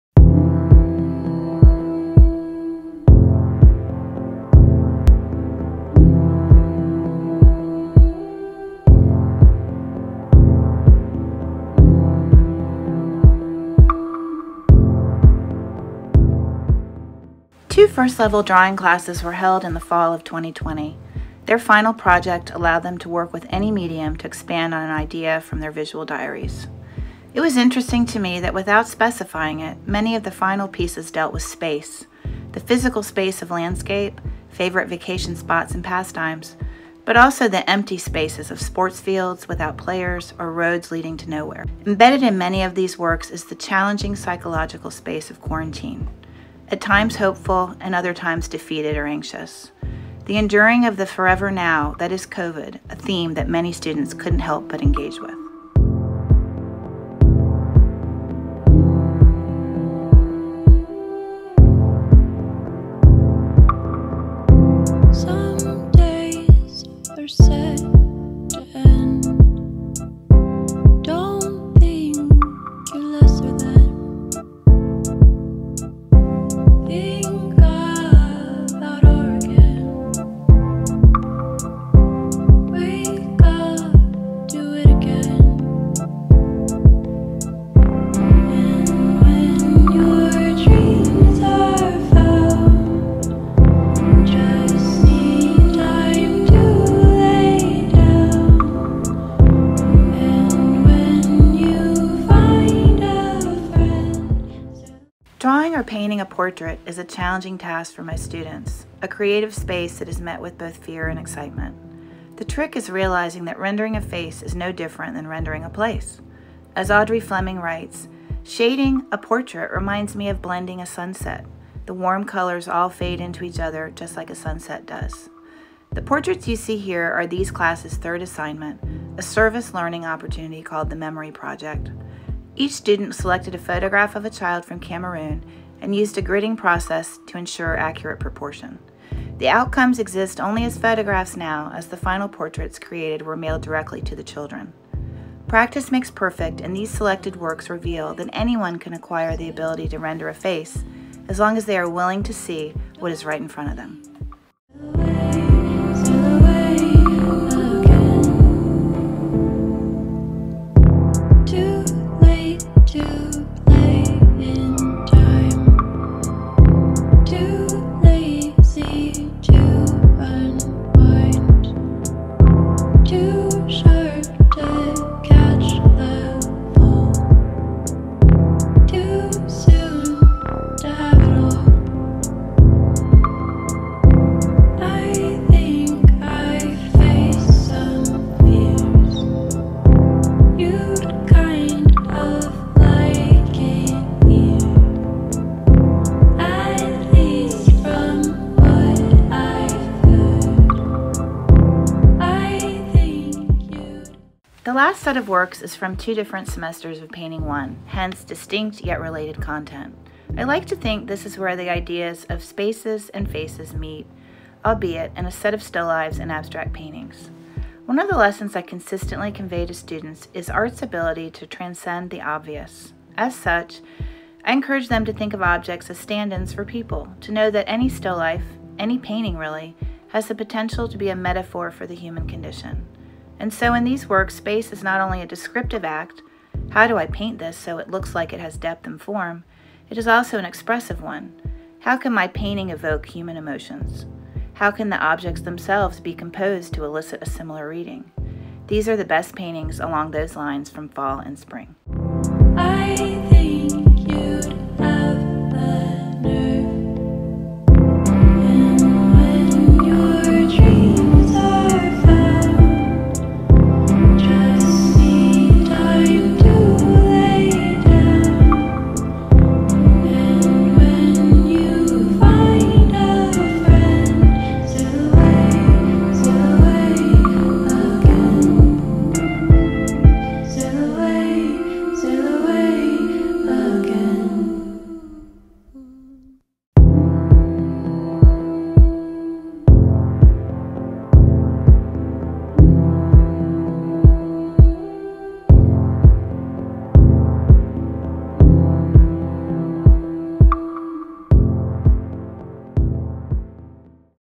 Art Walk 2021 Upper School Painting & Drawing
Selected works from Upper School Painting and Drawing Classes